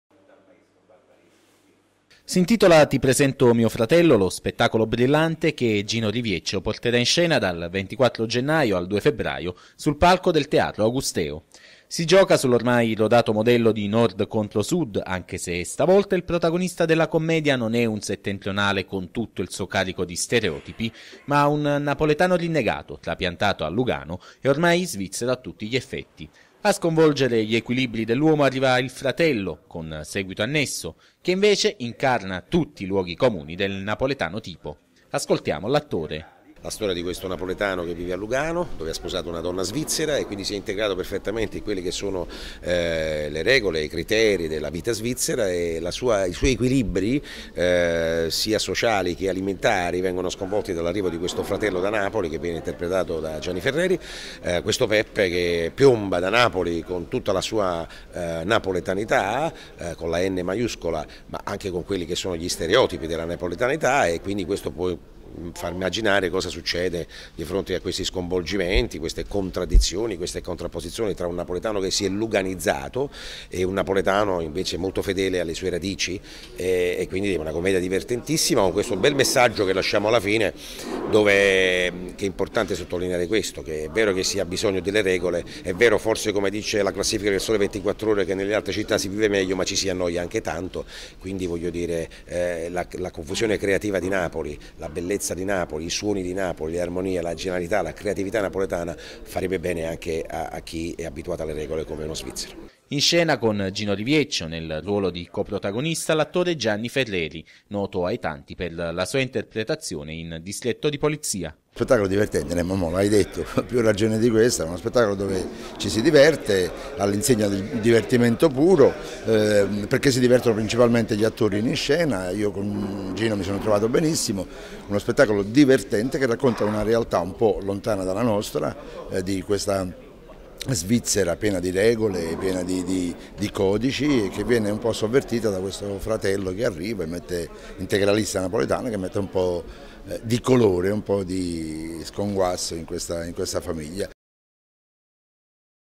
Napoli - "Ti presento mio fratello", nuovo spettacolo di Gino Rivieccio -2- (23.01.14)
- Napoli - "Ti presento mio fratello", il nuovo spettacolo di Gino Rivieccio in scena al Teatro Augusteo dal 24 gennaio 2014.

Una commedia scritta da Stefano Sarcinelli, Peppe Quintale e Gino Rivieccio, con Gianni Ferreri. Un testo attualissimo, che ci presenta una coppia che vive a Lugano, nel Cantone svizzero, apparentemente serena, con un bell'appartamento. Stefano è un avvocato napoletano cinquantenne, inserito perfettamente nelle abitudini del luogo da quando ha sposato Petra, che gestisce una galleria d'arte moderna. L'unico disagio  è che la coppia non riesce ad avere figli. L'arrivo inaspettato di Peppe, fratello di Stefano, ex cantante neomelodico, travolge la loro vita, contrapponendo due mentalità, due modi di vivere, e offrendo al pubblico momenti di comicità e riflessione. (23.01.14)